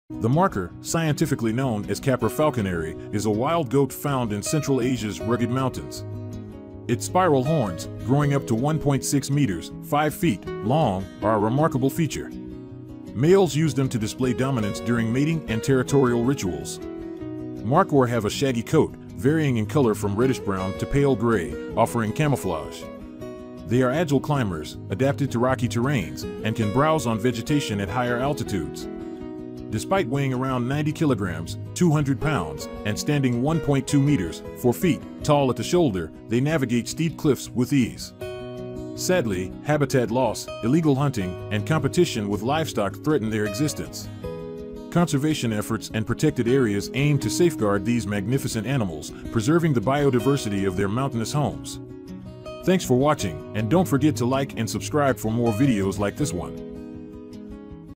Discovering the Enigmatic Markhor and Its Striking🐐😱
Meet the Markhor: Majestic Master of the Mountains. The Markhor, with its striking spiral horns and captivating gaze, is a true symbol of grace and resilience. This enigmatic creature inhabits the rugged mountain ranges of Central Asia, effortlessly navigating steep cliffs and rocky terrains. With its remarkable agility and stunning appearance, the Markhor captivates the hearts of wildlife enthusiasts worldwide. Discover the secrets of this elusive mountain dweller as it roams its natural habitat, showcasing its majestic presence and embodying the spirit of the wilderness.